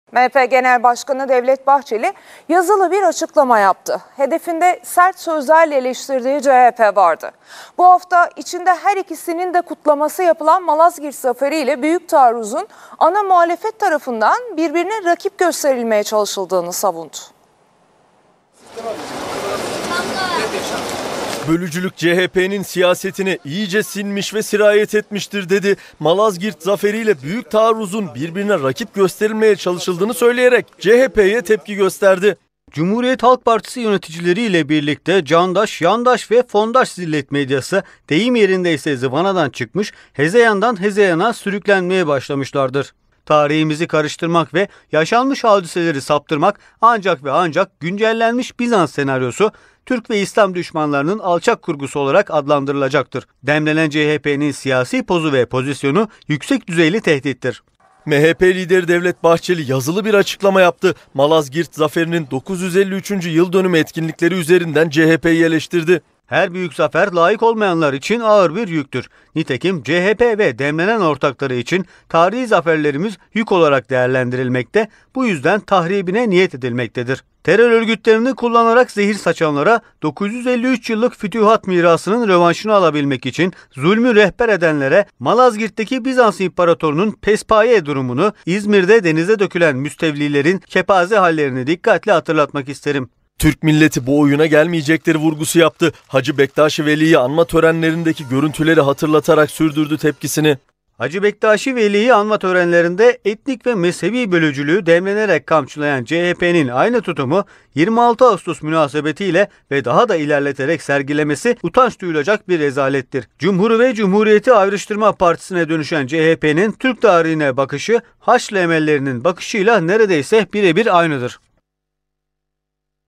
CHP’nin aynı tutumu 26 Ağustos münasebetiyle sergilemesi utanç duyulacak bir rezalettir.
Hacı Bektaşi Veli’yi anma törenlerinde etnik ve mezhebi bölücülüğü DEM’lenerek kamçılayan CHP’nin aynı tutumu 26 Ağustos münasebetiyle ve daha da ilerleterek sergilemesi utanç duyulacak bir rezalettir.